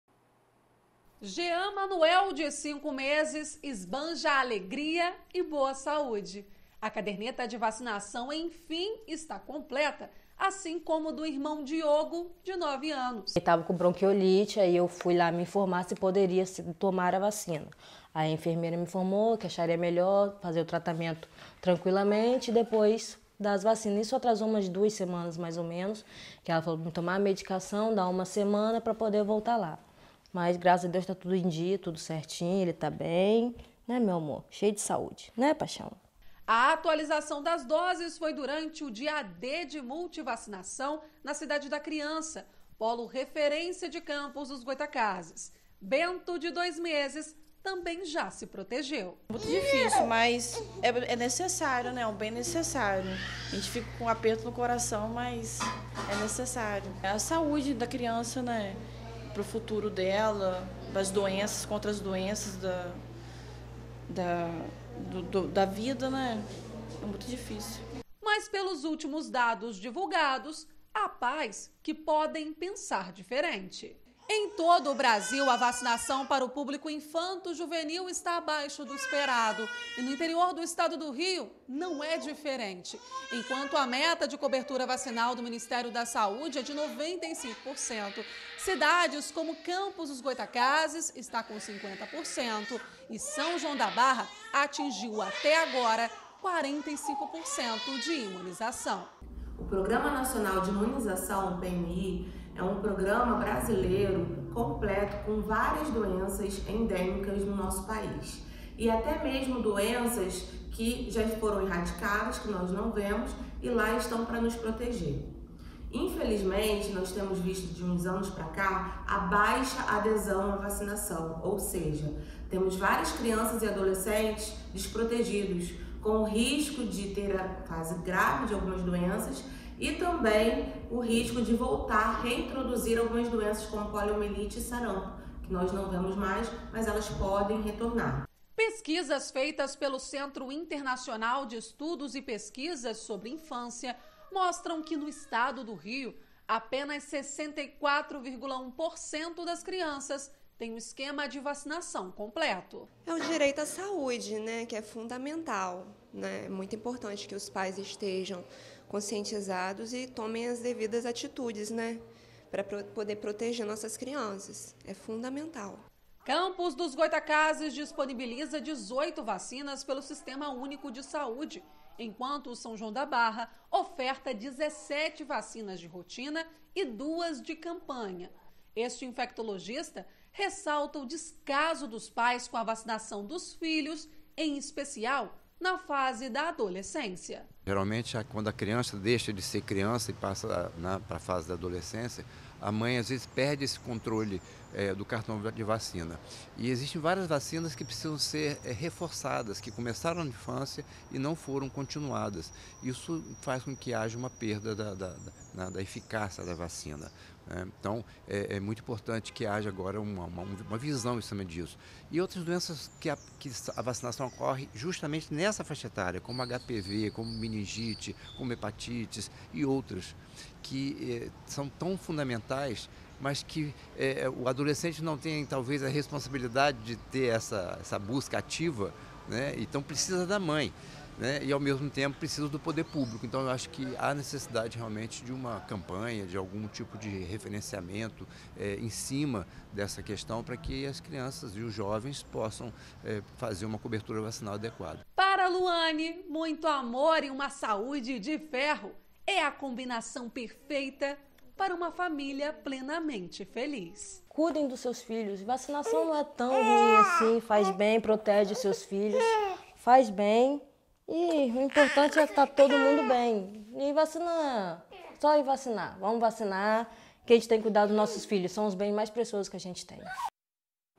Baixa cobertura vacinal vem sendo desafio para o sistema de saúde
A vacinação brasileira, que é modelo para o mundo, enfrenta desafios diante da baixa cobertura. Setembro, mês em que o programa nacional de imunizações completa 50 anos, os números estão em queda.